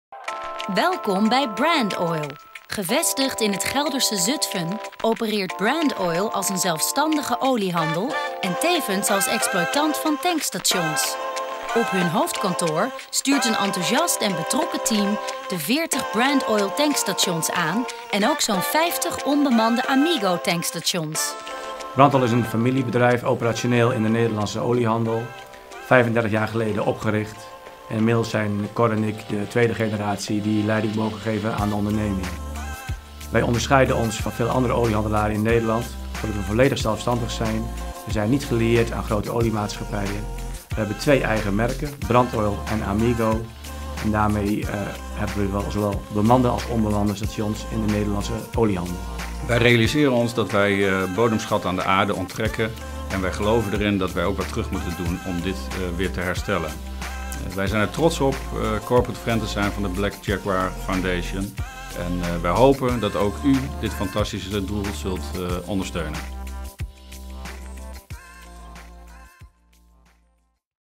092/600 BrandOil - in support of the Black Jaguar Foundation
Cor Brandsma & Arjen Sietsma, owners of Brand Oil and Amigo Tank stations in support of the Black Jaguar Foundation.